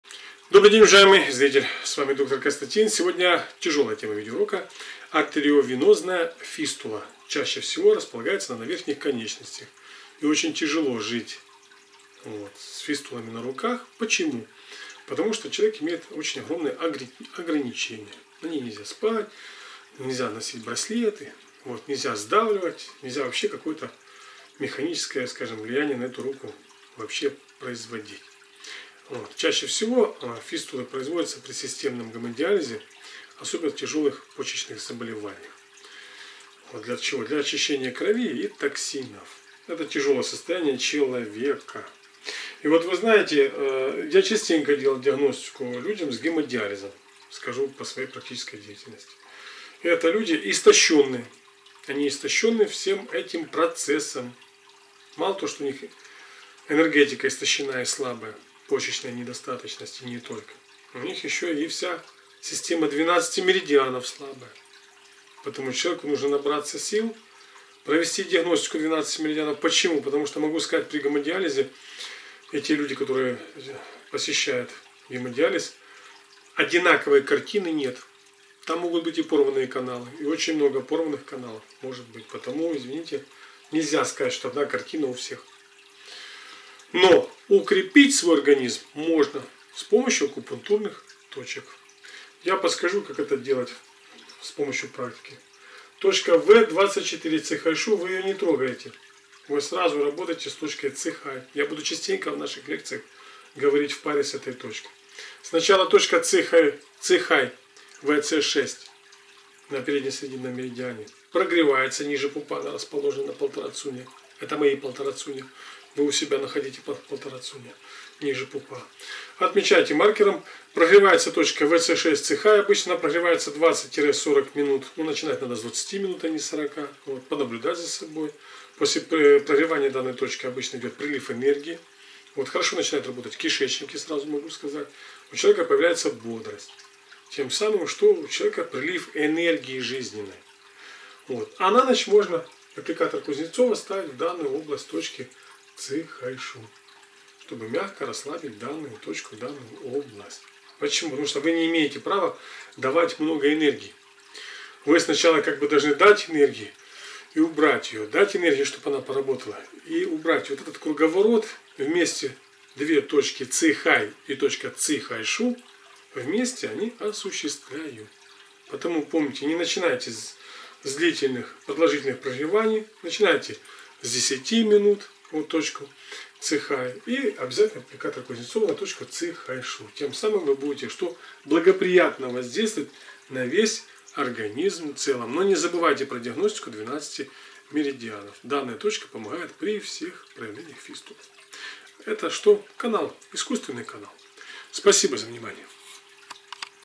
Артериовенозная Фистула
Восточная медицина в помощь. Видео-Урок Иглотерапевта Доктора Константина.